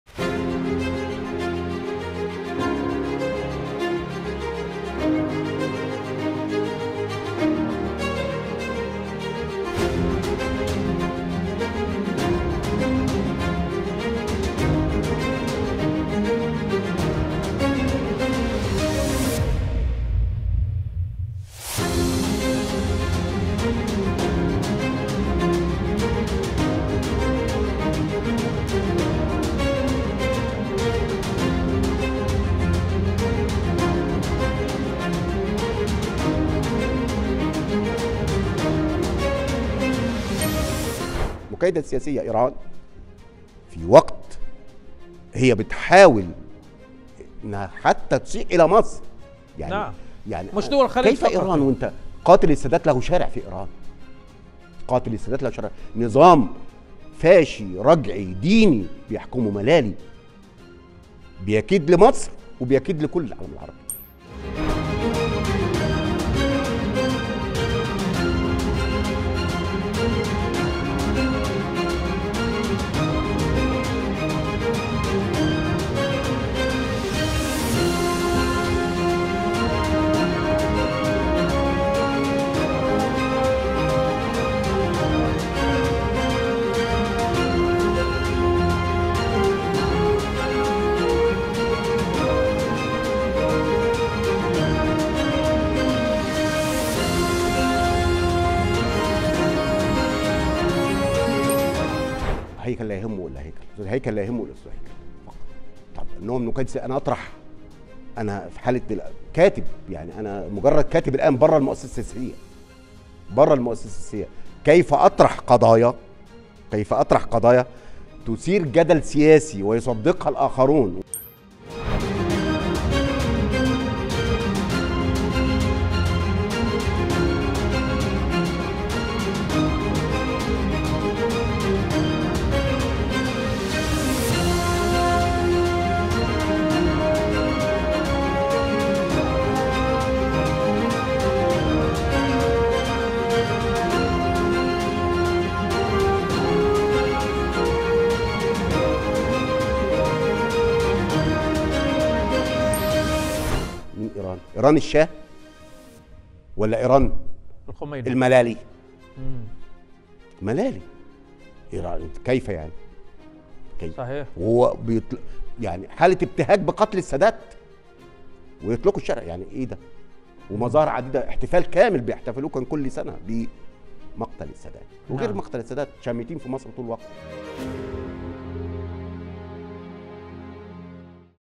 الباحث والكاتب أحمد سعيد المصري يعيد قراءة آراء محمد حسنين هيكل السياسية في إيران
أطروحاته عنها وصفت بالمثيرة للجدل.. الباحث والكاتب أحمد سعيد المصري يعيد قراءة آراء محمد حسنين هيكل السياسية في إيران
لمعرفة أحدث المستجدات.. عبر البث المباشر من هنا ⤵
وبإمكانكم متابعة العربية على منصات التواصل